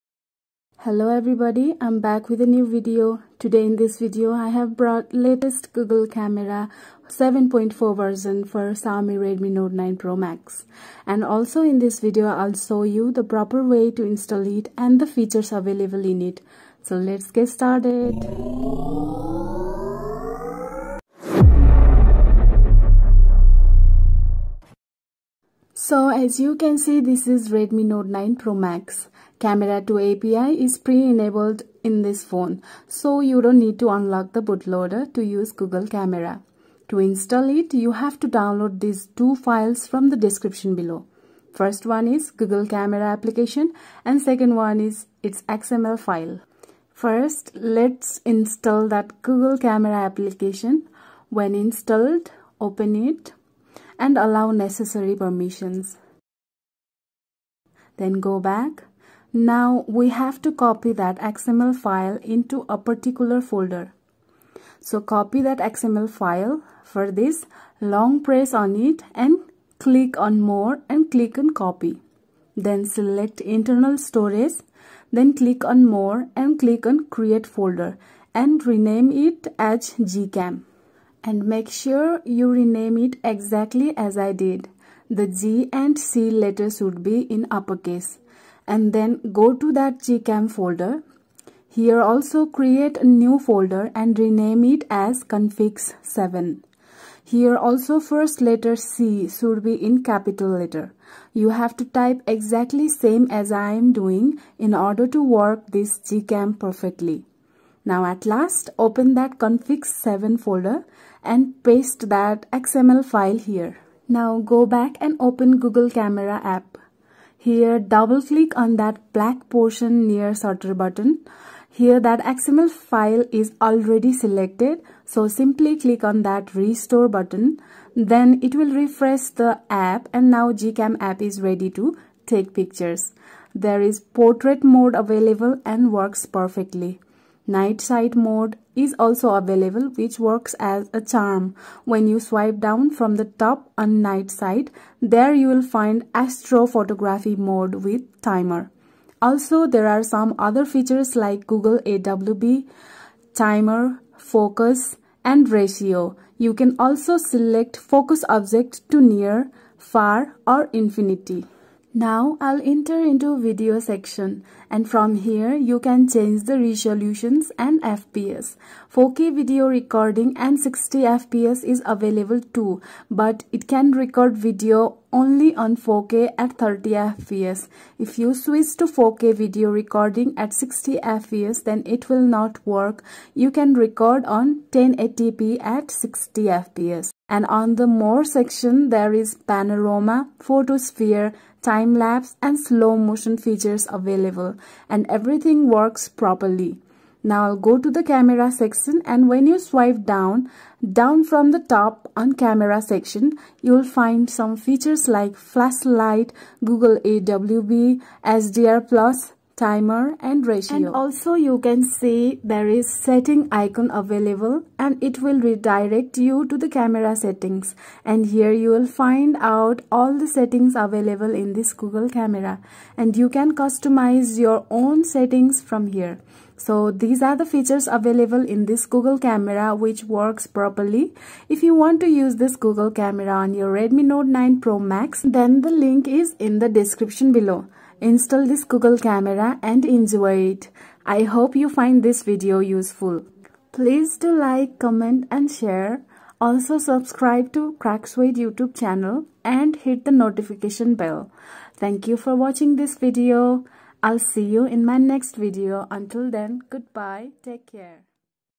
Google Camera For Xiaomi Redmi Note 9 Pro Max 😍
Welcome to Craxoid. Today in this video I will show you the review and installation process of google camera for Xiaomi Redmi Note 9 Pro Max. Watch this video if you want to know about latest google camera 7.4 for Redmi Note 9 Pro Max